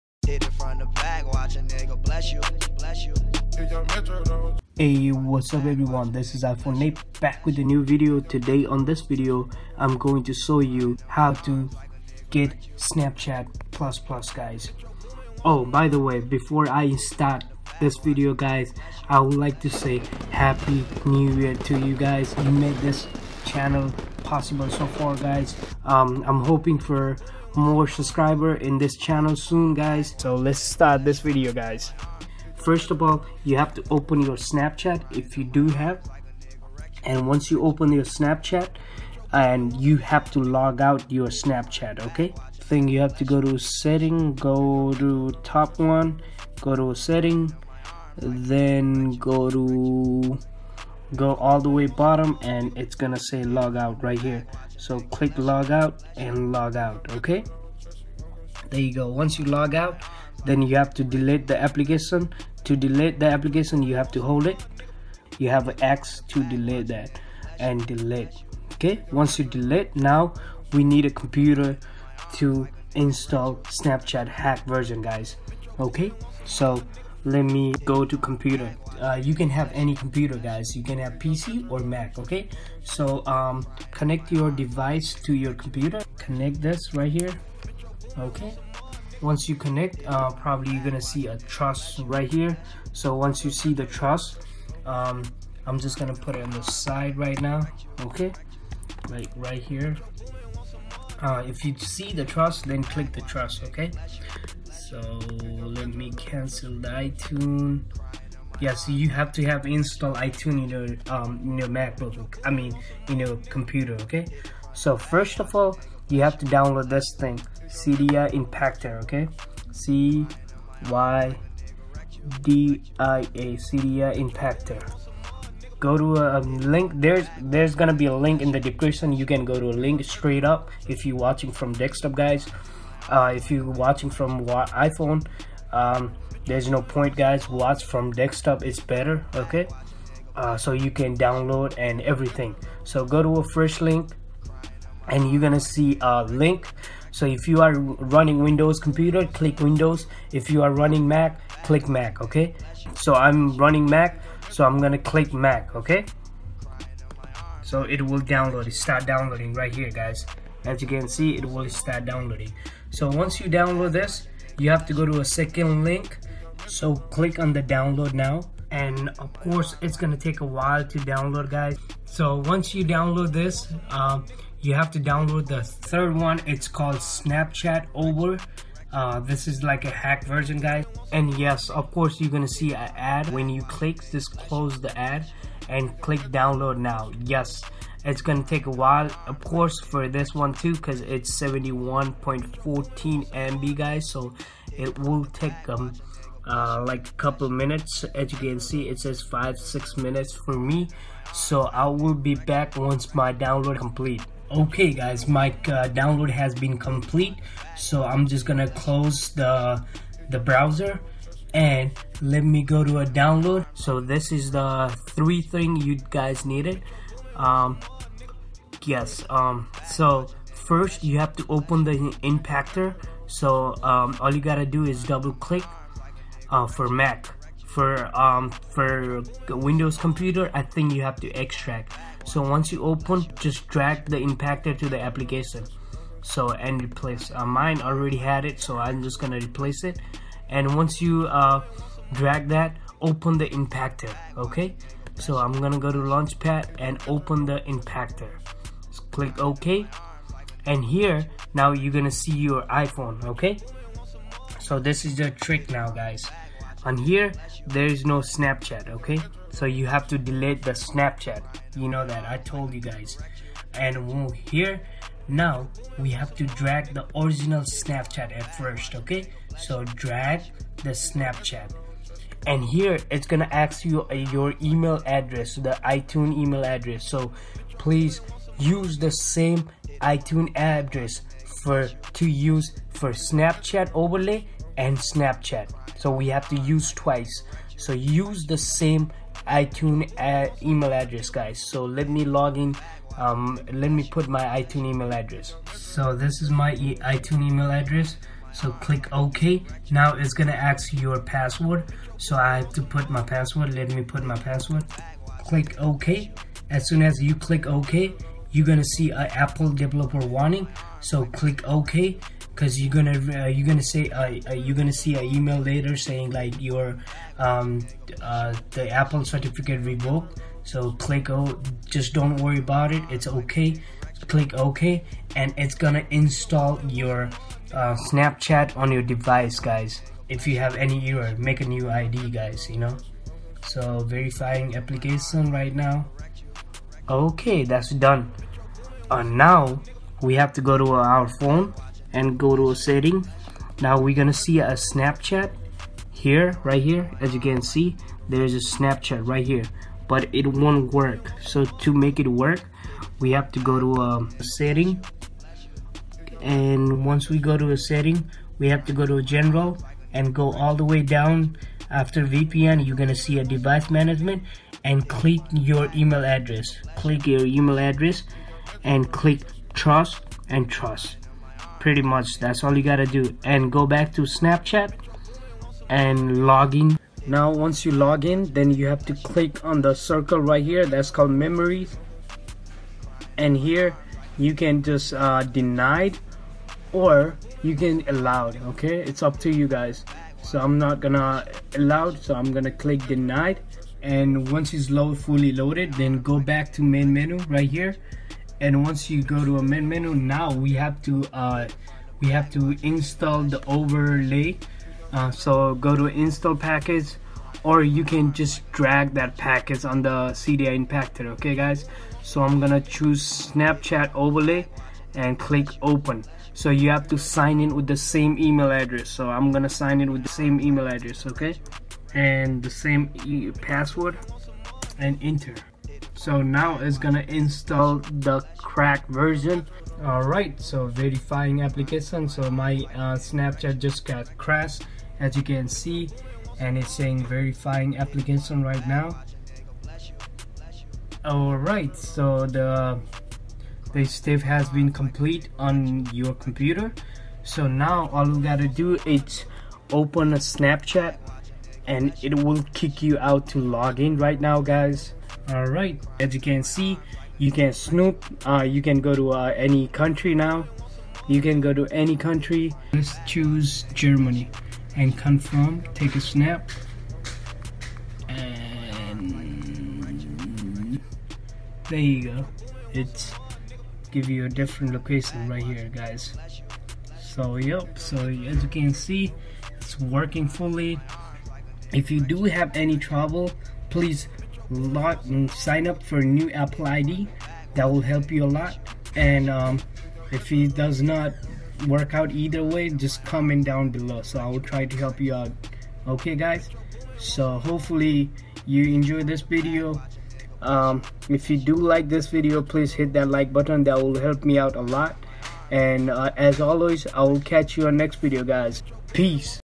How To Install Snapchat++ Hack On iOS 10/9 & Below For Phone, iPad, iPod- No Jailbreak Required
This is a old method so it might not work for you ... :( wait for new version hack.

Rate if You Like This Video

On This Video I will show you How To install Snapchat ++ HACKS 2017 On iOS 10/9.3.3/9 & Below For Phone, iPad, iPod Touch. This method fix Could Not Connect too.